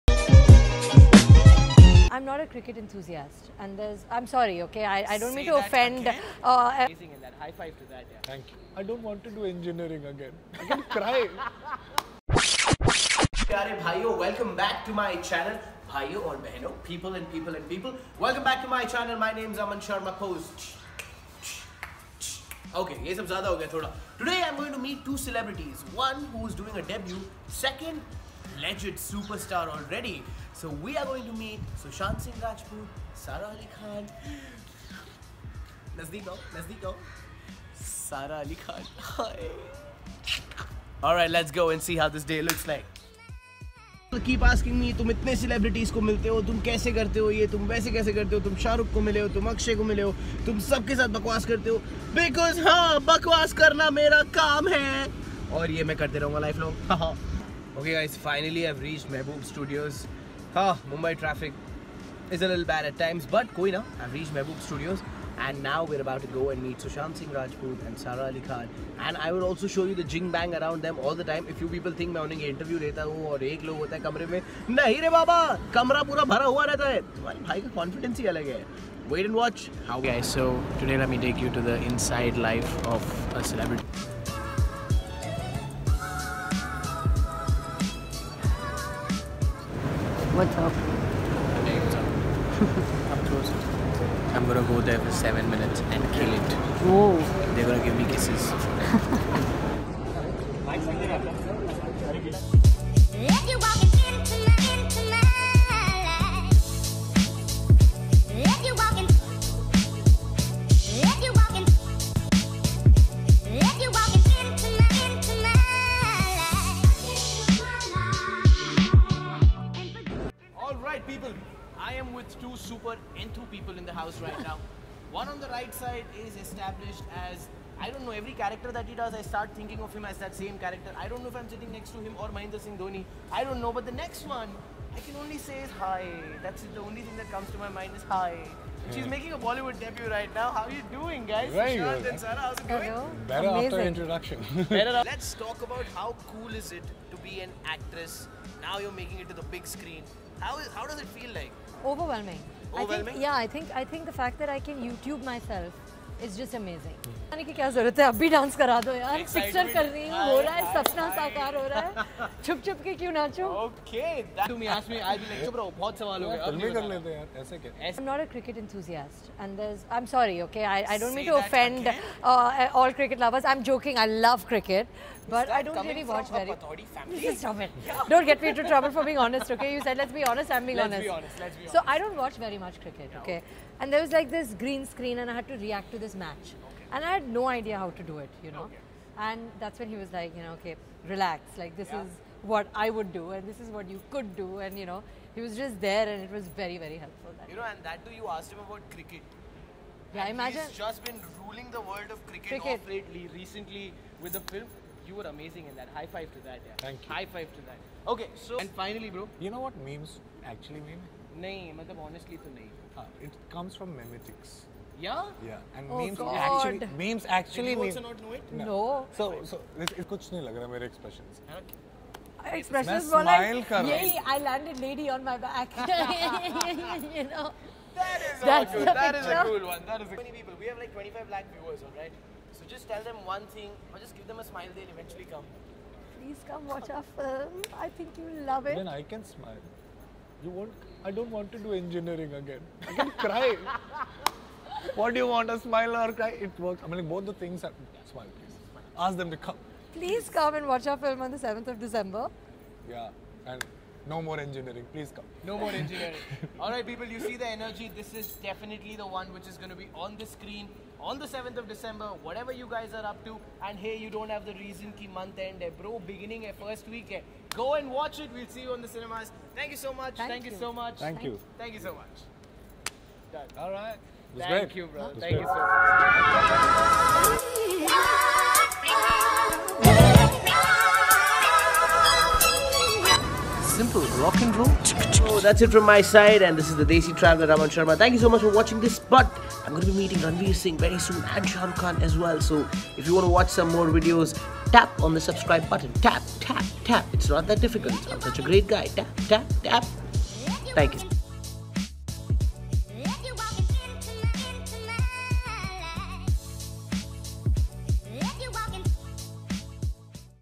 Gyaan baba Sushant and Sara - Kedarnath Interview with Aman Sharma
You know your life is amazing when you get to meet superstars but what makes it all the more crazy is the learning that comes with it! Sushant clearly is a knowledge hub and is just amazing at what he does and Sara on the other end is a livewire. Watch me have kickass masti with the two stars of Kedarnath!